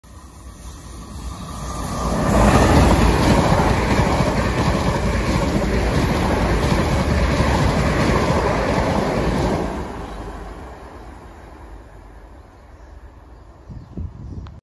Class 444 Desiro | 444034 + 444015 | South Western Railway
A different pairing of Class 444’s speed through.